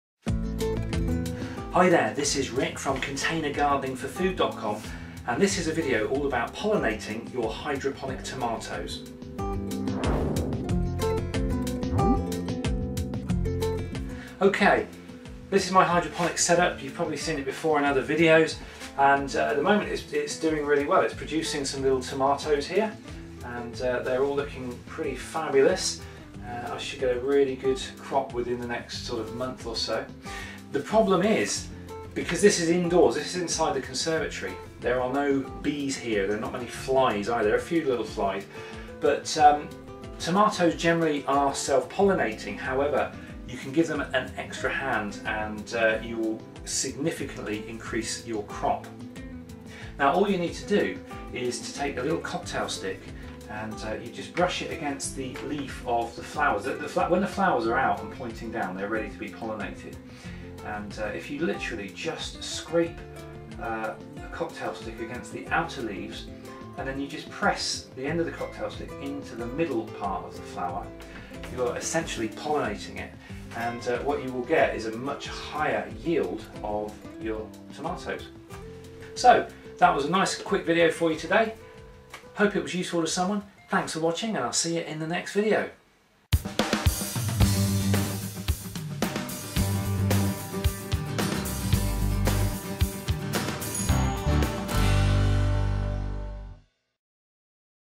Pollinating Tomatoes (hydroponic)
A quick video showing you how I pollinate my hydroponic tomatoes